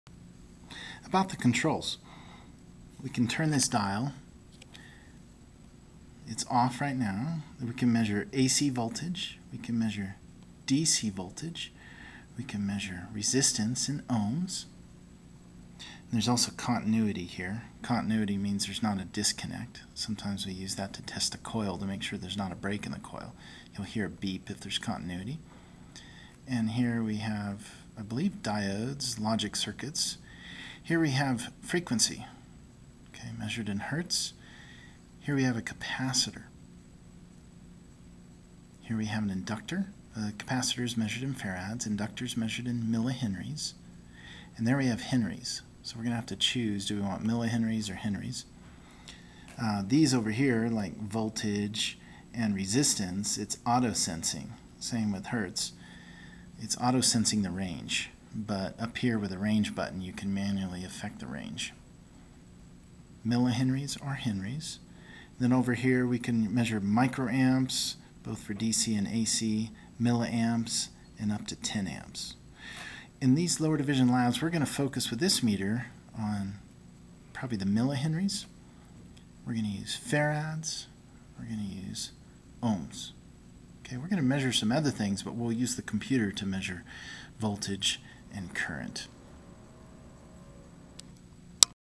DMM-01- Measurement Selector for DMM Amprobe 37XR-A
Measurement Selector: Measurements that are available on the Amprobe DMM (Digital Multimeter)